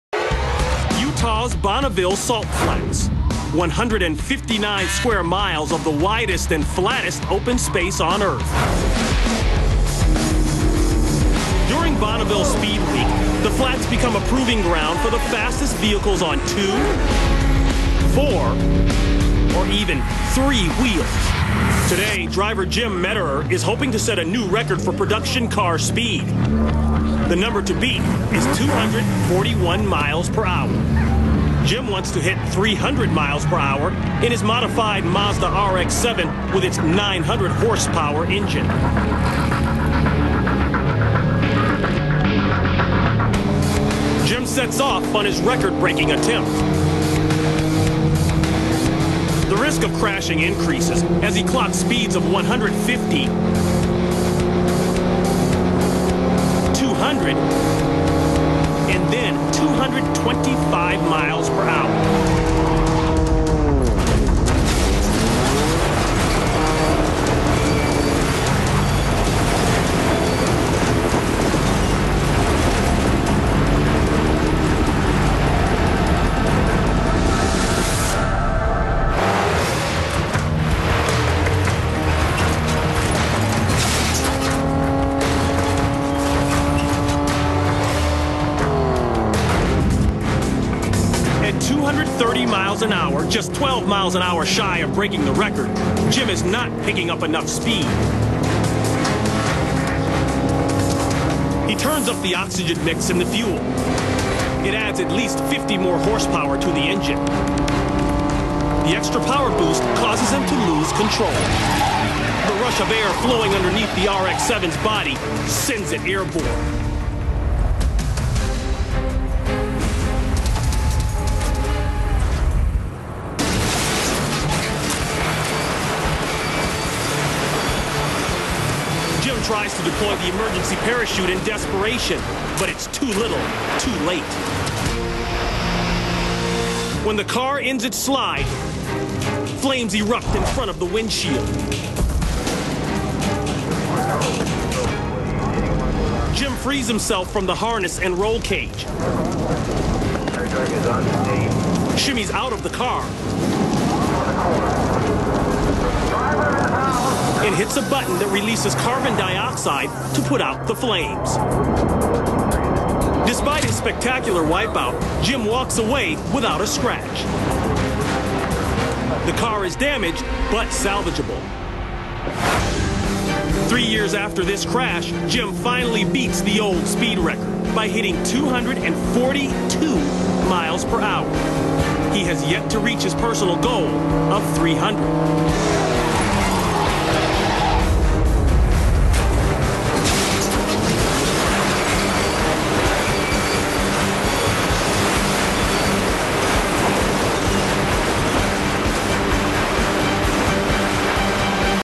Mazda RX-7 Crash at 225 MPH!!
Bonneville Salt Flats:

3rd gen Mazda RX-7 race car trying to beat the land speed record for a production vehicle.

At this speed, the tire patch that is touching the ground is LESS than the width of your index finger across each tire.

Furthermore each patch is encountering dust and small sand-like particles often.  If you could keep the pace with this car and remain standing next to it, you could reach over and push it away from you sideways with little effort.

The announcer claims that an additional burst of oxygen in the fuel causes a loss of control, but in all likelyhood, the wind just changes ever so slightly, or began to blow (even lightly) and it was actually this that nudged the cars nose off by just enough to start a flat spin.

Yes, a flat spin, like an airplane goes into!

No rudder on this vehicle, so no control!